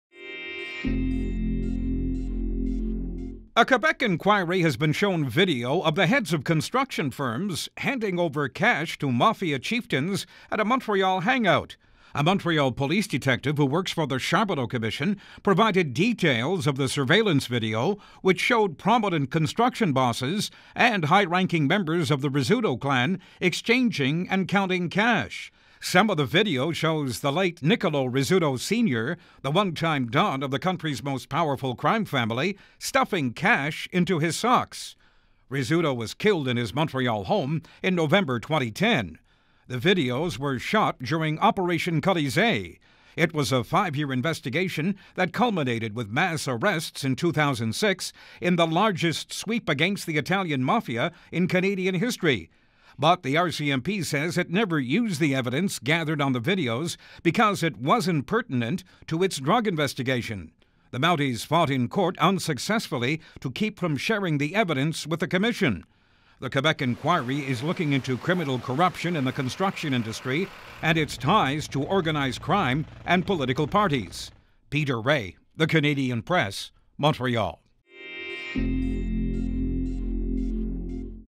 Corruption inquiry sees video of mob boss receiving cash
A corruption inquiry was shown surveillance video Wednesday of construction bosses handing over cash to the Mafia. The Charbonneau Commission is looking into allegations of criminal corruption in the construction industry.